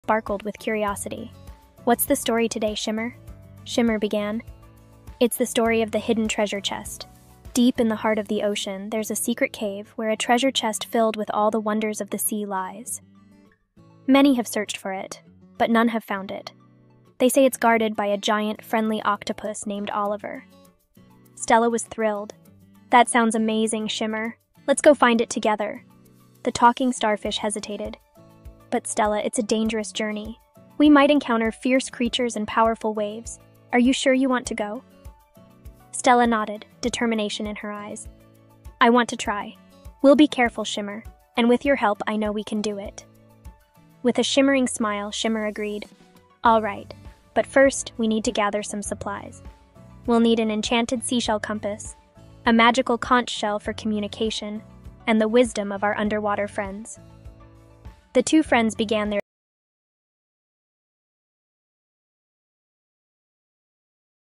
Stella and the Talking Starfish | animationmeme | Mermaid tail | English fairy tales
Stella and the Talking Starfish is a very beautiful story in the water.In this Stella and the Talking Starfish animation, Stella discovers a magical mermaid tail while on a family vacation. She quickly befriends the mermaid and the two of them embark on a fun journey together!

Stella and the Talking Starfish is an enchanting English fairy tale that is perfect for children. Watch as Stella and the mermaid explore the ocean and experience all sorts of exciting adventures together! Stella and shimmer try to find the treasure box in the deep sea. I hope you like this story. Have Fun !